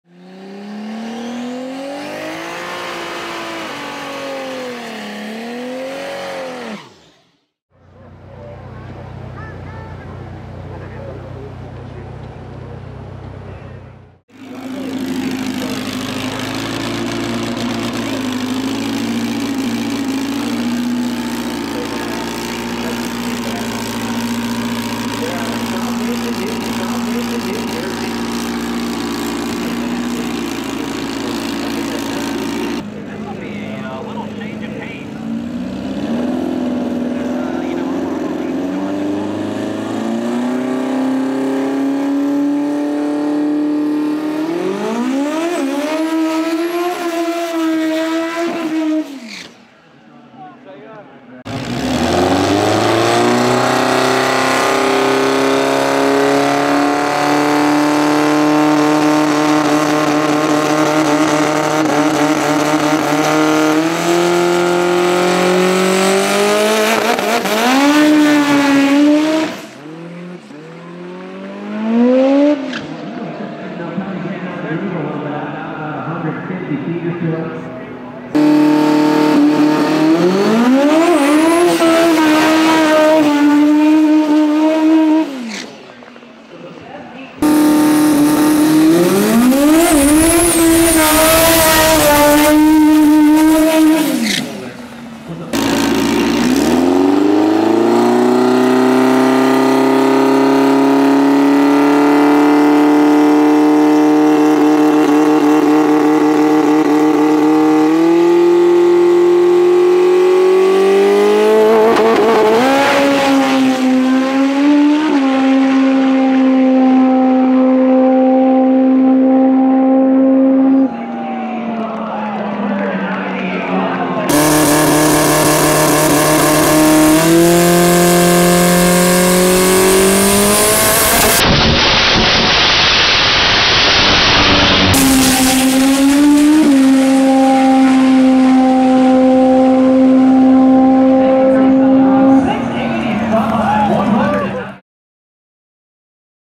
122ci of Fury! 6sec 4G63 Dragster
The Extremepsi/Turbo4.com 4G63 dragster made a couple of passes at the 2015 DSM/EVO/GTR Shootout. This is the Quickest & Fastest 4G63 in the world!